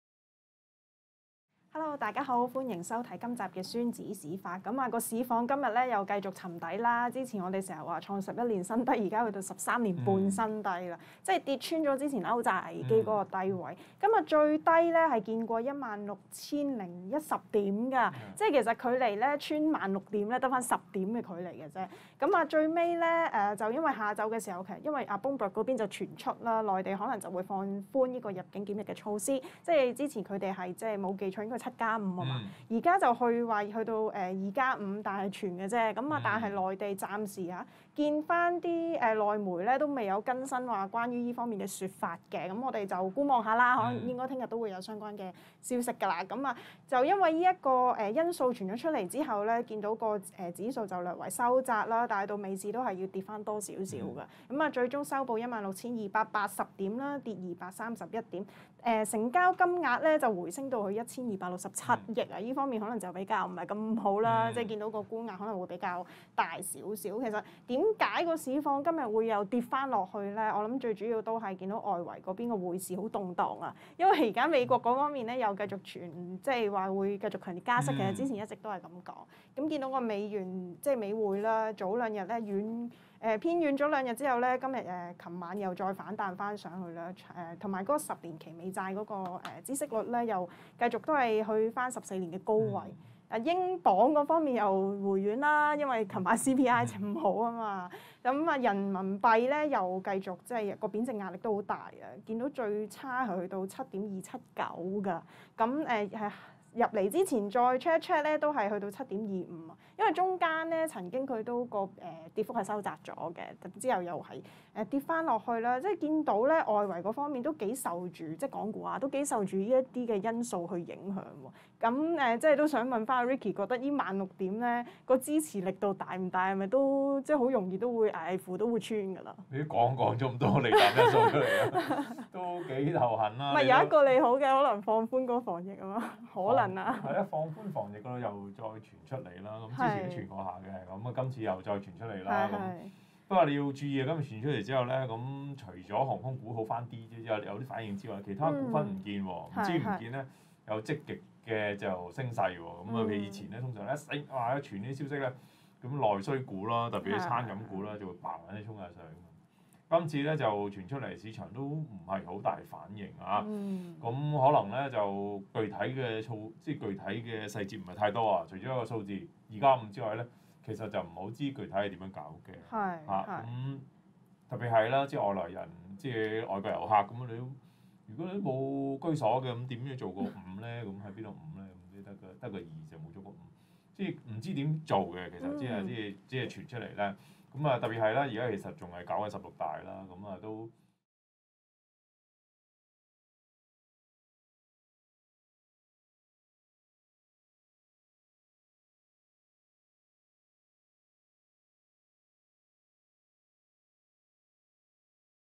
【孫子市法】債息破頂盈利降 中概急挫萬六危(精華片段)
iMoney【孫子市法】債息破頂盈利降 中概急挫萬六危(精華片段)

其他股評時段請立即收看足本重温
00:20 恒指
01:20 外幣匯率
19:10 中概股
20:00 9988 阿里巴巴 1024 快手
20:10 9999 網易 9888 百度
27:30 科技股
30:30 芯片半導體股
31:25 1347 華虹半導體 0981 中芯國際
45:10 Q&A

【請將 iMoney智富雜誌 專頁設定為 "搶先看/See First"】